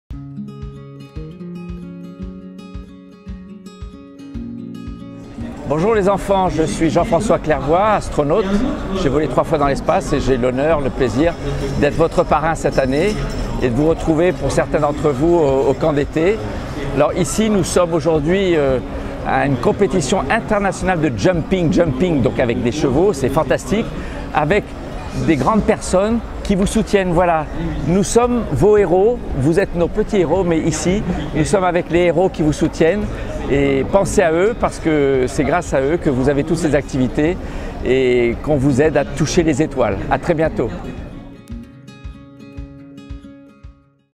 Jean-François Clervoy en direct du PEJ
En direct du Longines Paris Eiffel Jumping, notre parrain de l'année, l'astronaute Jean-François Clervoy, souhaite partager une pensée aux enfants de L'ENVOL !

Il sera à nos côtés pendant les séjours d'été pour partager ses aventures et rendre l'espace plus accessible aux enfants.

Envie de profiter des séjours de L'ENVOL ?